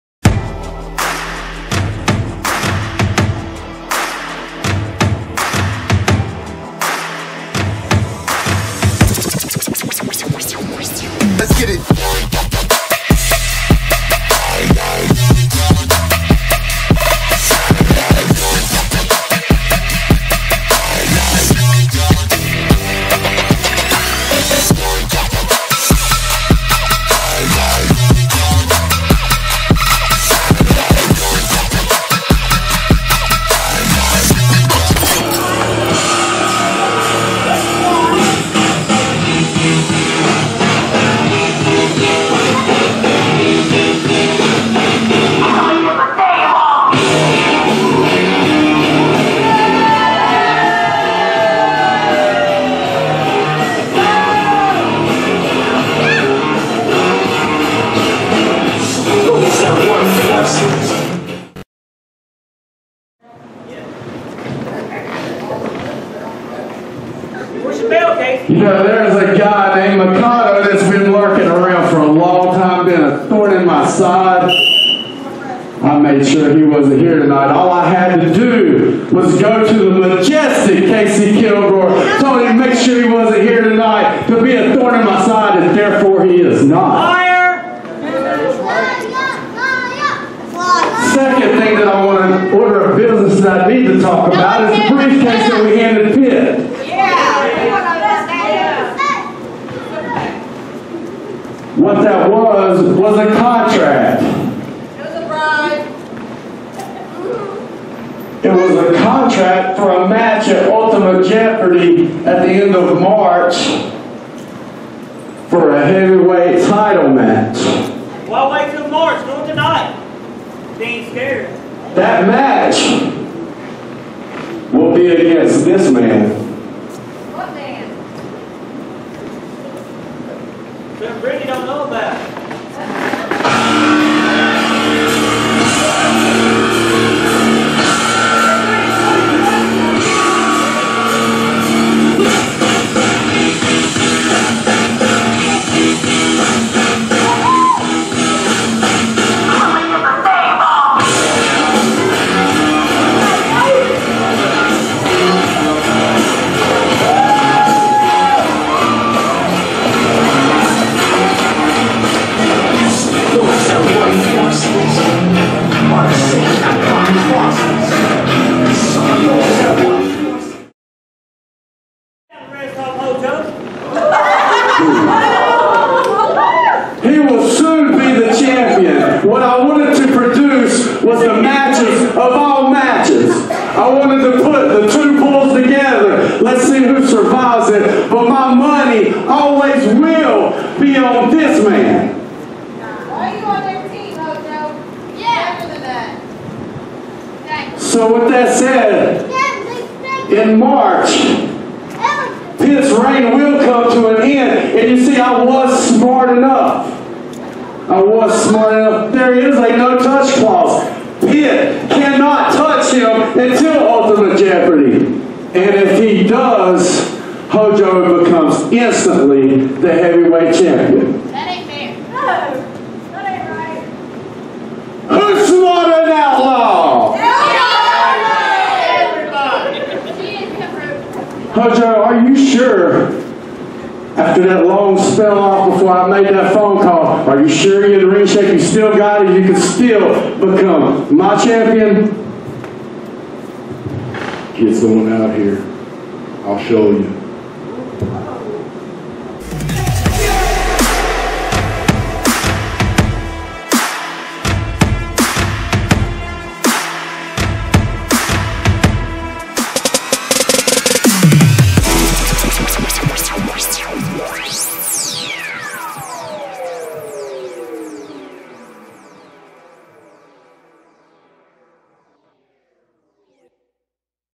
MWF Owner, "Outlaw" Keith Matthews Bring's Back "The Devil" Hojo Devlin! - 01-27-18
The Owner of the MWF, "Outlaw" Keith Matthews reveals what was in the briefcase that he gave to "The Alley Rat" PITT the show before...a contract for PITT's next opponent he will face at Ultimate Jeopardy.

Hojo "The Devil" Devlin!